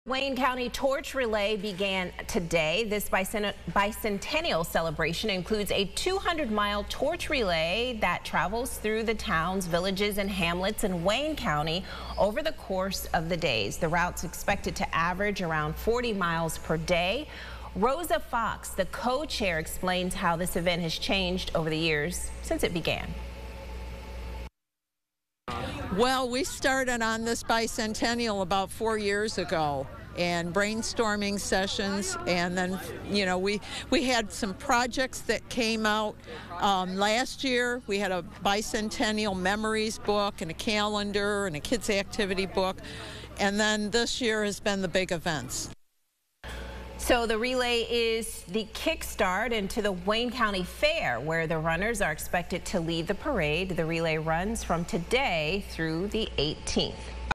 Wayne County Torch Relay kicks off
The 200-mile torch relay is part of the county's bicentennial celebration. It will travel through the towns, villages and hamlets in Wayne County. The route is expected to average around 40 miles per day through August 18.